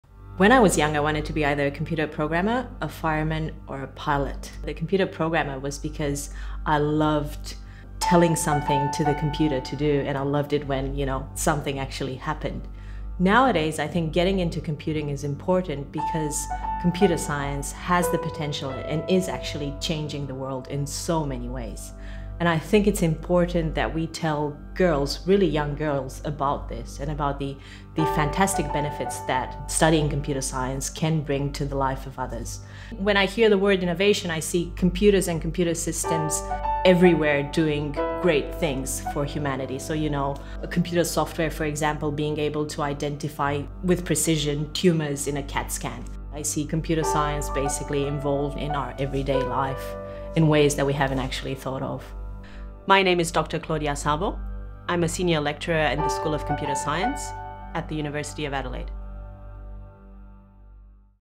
Dr Claudia Szabo - Winner of the 2017 STEM Educator of the Year – Tertiary Teaching award
Congratulations to  Dr Claudia Szabo, University of Adelaide, winner of the 2017 STEM Educator of the Year – Tertiary Teaching award.